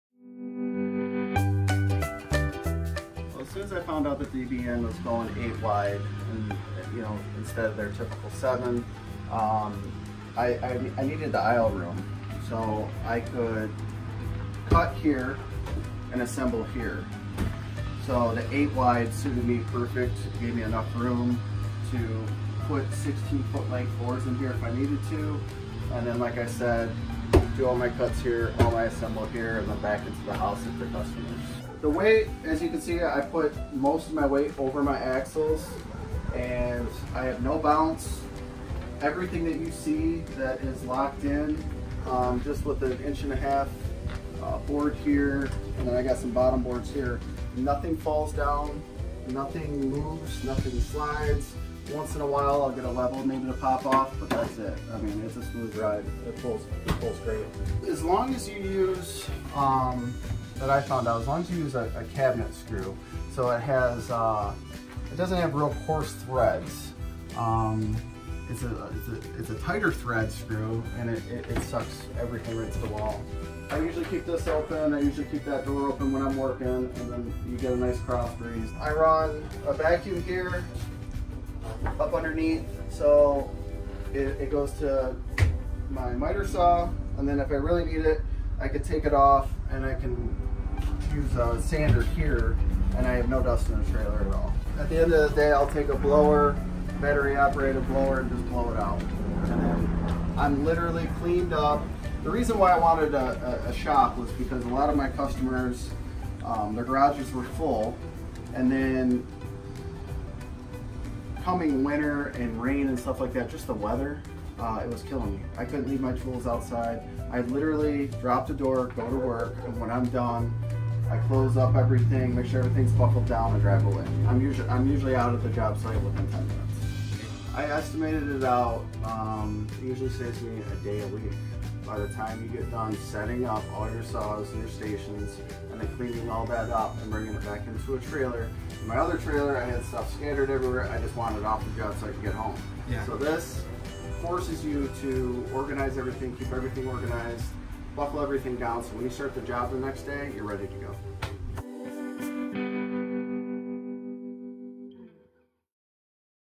LEGEND PREMIUM TRAILERS Contractor Ryan 8wide DVN
Aluminum 2" X 4" Tube Perimeter Frame
Aluminum 1" X 1.5" Tube Wall Cross Members 16" On Center
Aluminum 2" X 3" Tube Floor Cross Members 16" On Centre
Aluminum 1" X 1" Tube Radius Roof Bows 24" On Centre W/Stringers
3" X 6" Extruded Aluminum Triple Tube Tongue (32")
3500LB Dexter Torsion Axles W/ E-Z Lube System
Screwless Aluminum Exterior Skin
Seamless One Piece Aluminum Roof
Aluminum Wheels W/ 15" Radial Tires
3' V-Nose
ATP Wheel Boxes & Flares
Stainless Steel Door Hardware W/ Key Lockable Hasp (Ramp Only)
Aluminum Hinges On Barn Doors
30" RV Side Door W/ Flush Lock & Aluminum Holdback With Black Frame
Lighted Side Entry Door Grab Handle
24" ATP Stone Guard
16" Polished Mesa Skirting
Bright Polished Radius Roof Rails & V-Nose
Led Exterior Lighting
(2) 12V Rectangle Led Interior Dome Lights W/ Wall Switch
3/4" Engineered Wood Flooring
3/8" Engineered Wood Interior Wall Covering
14" X 14" Manual Crank Roof Vent
Bright Polished Accent Divider Strip W/Legend Decal
2000LB Top-Wind Set Back Jack W/Foot
2 5/16" Coupler
(4) 1000LB D-Rings Installed
Self Mating Rear Ramp Door W/ Spring Assist W/Stainless Steel Door Hardware W/Key Lockable Hasps
Limited 10 Year Axle - Limited Lifetime Structural - Limited 25 Year Floor - Limited 25 Year Roof Warranty

for more information or come in to our Strathroy location, just 20 minutes outside London off the 402!